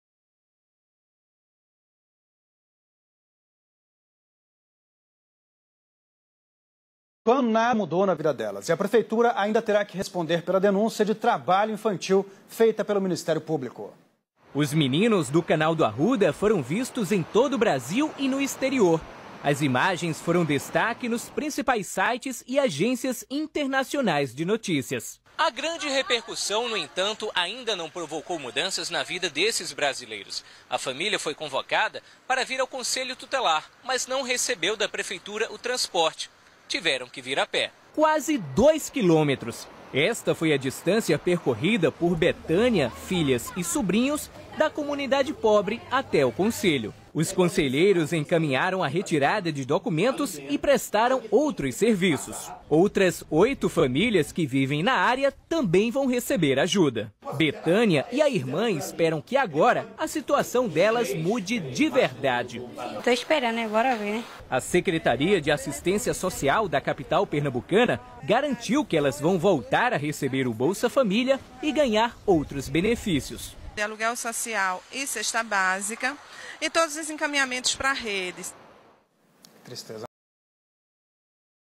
Crianças no meio do lixo, no Canal do Arruda em Recife PE
Refice capital pernambucana, administrada pelo prefeito Geraldo Julio (PSB), aliado político do governador de Pernambuco  Eduardo Campos (PSB), pre-candidato a presidente nas eleições de 2014. O que será do Brasl ?